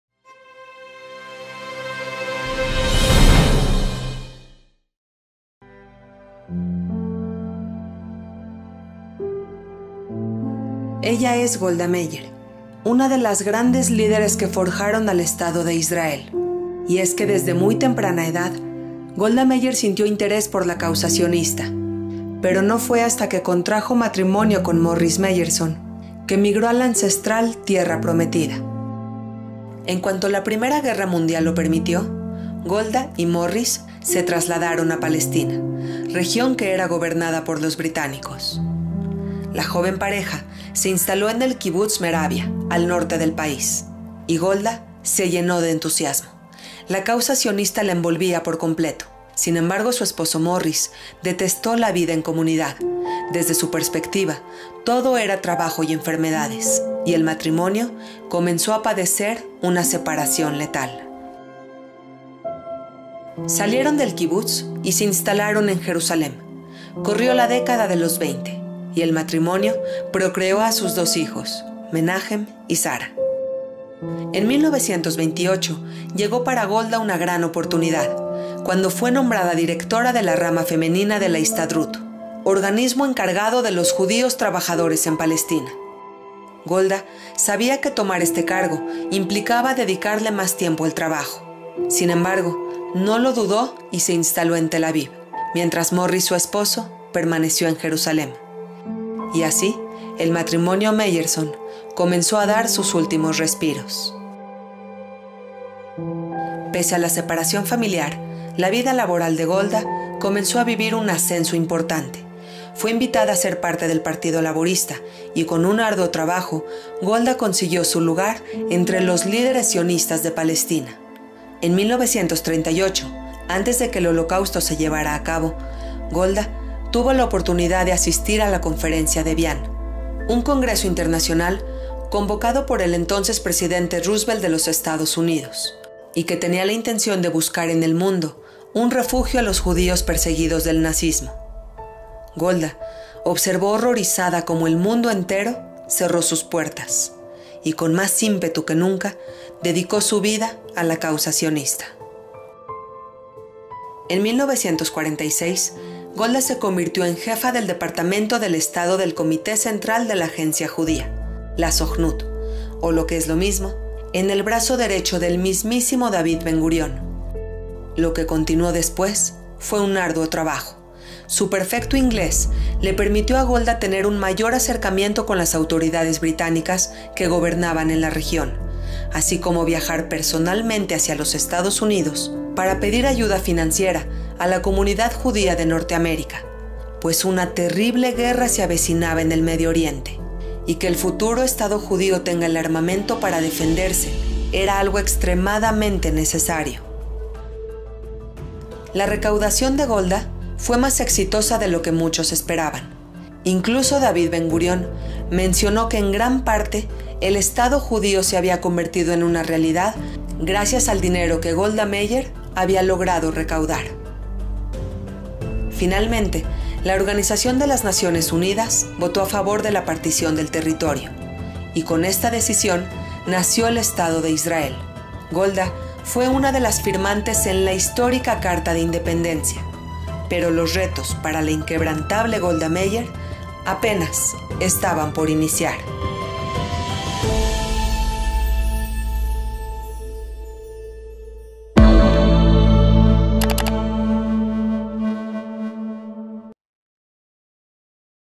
Los primeros años del sionismo de Golda
Ella es Golda Meir, una de las grandes líderes que forjaron al Estado de Israel, y es que desde muy temprana edad Golda sintió interés por la causa sionista, pero no fue hasta que contrajo matrimonio con Morris Meyerson que emigró a la ancestral tierra prometida.